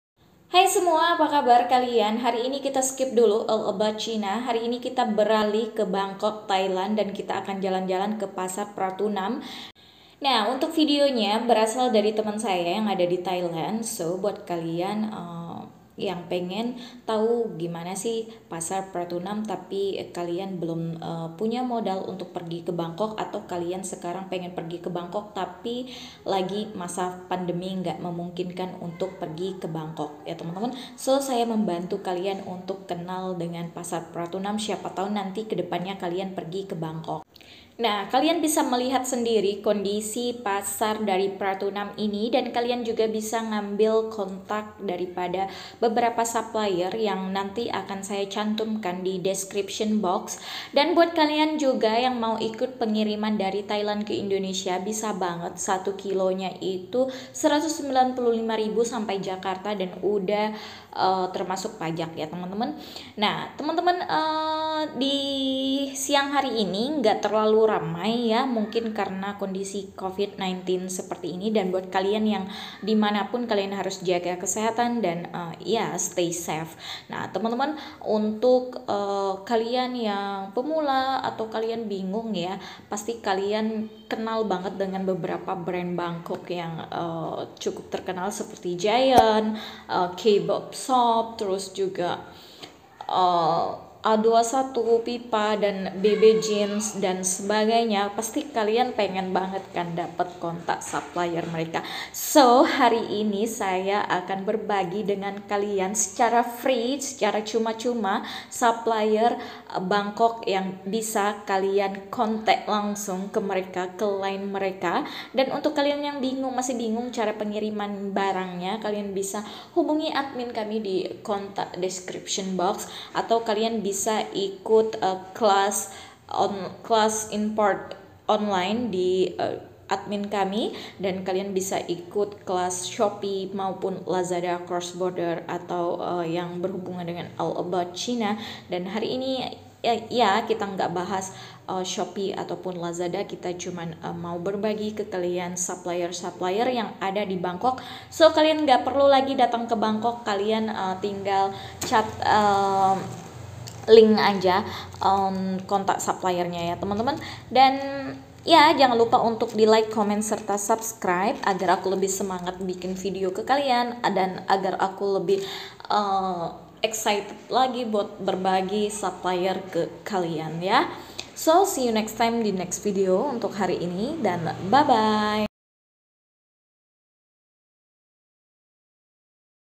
Ini Dia Daftar Suplier Bangkok Pratunam Market|Ana Maulida
INI DIA DAFTAR SUPLIER TERKENAL DI BANGKOK YANG BIASA DIJUAL PENJUAL BATAM MAUPUN SURABAYA

A21 SHOP (a21shop2559)
GI-ANTS ( @gi-ants )
Nan Fashion Shop ( pra-nanja )
Coco Paradise Shop accesories ( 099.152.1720)
Lamer Shop ( @Lamer.brand)
Yinde Shop(QR Code)
Chocolate Shope (QR Code)
Cristal Shop( Tas dan sepatu QR code)

Untuk QR CODE silahkan pergi kewhatshapp kami ya guys!

Belajar shopee crossborder
(Wechat id zahnia01)

The shipment to Indonesia can't be by shopee anymore
As for those of you who want to rip by cargo as well as a cheap minimum right now
Pengiriman ke indonesia by cargo juga murah minimum 5kg

Dropshipimport